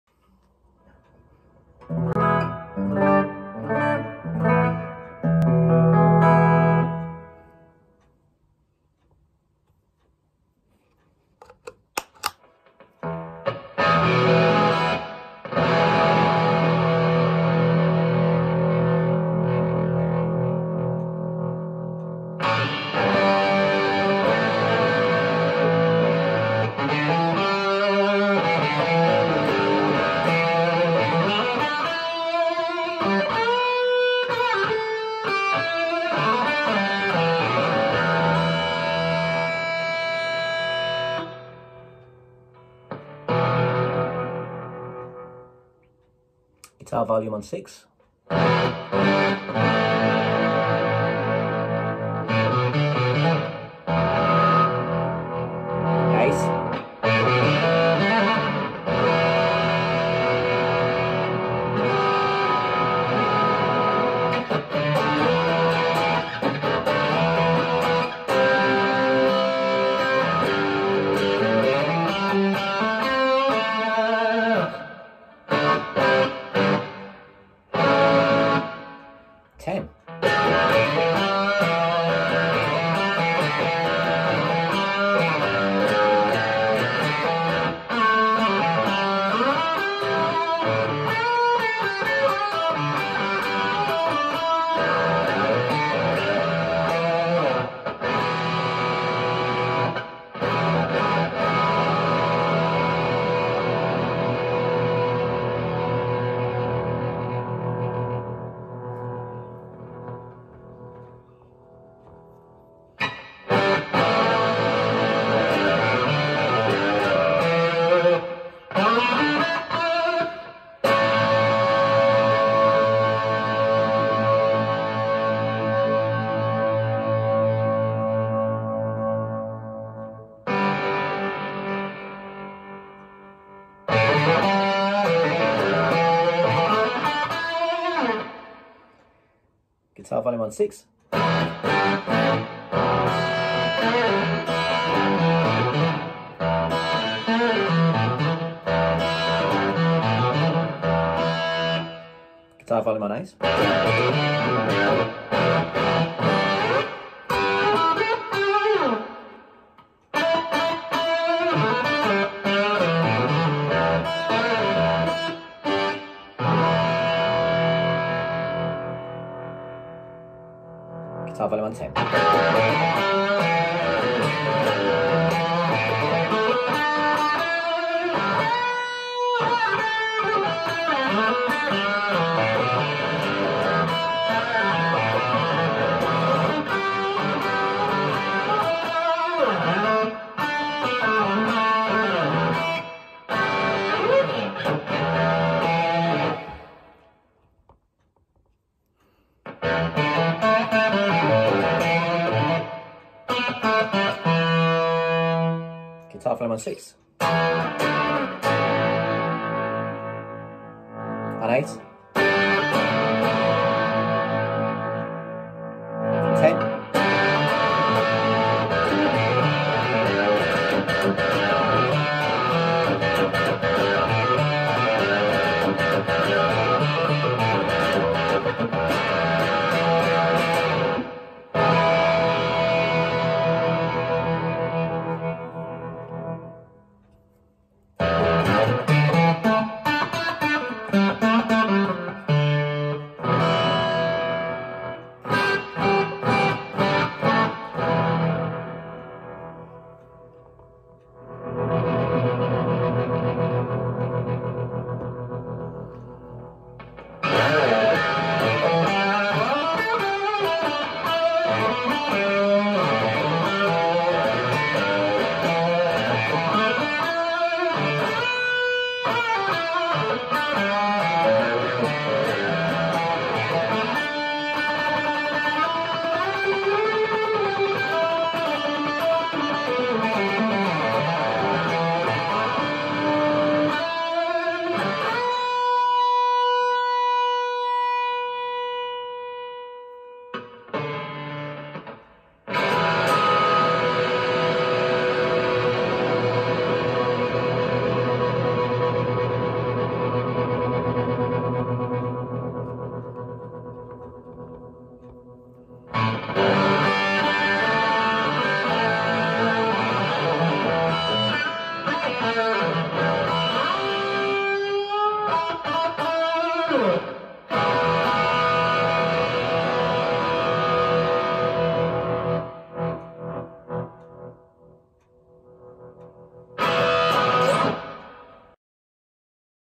Creepy fingers HornSkewer
Brads version of the 65’ zonk machine.

My favourite zonk clone to date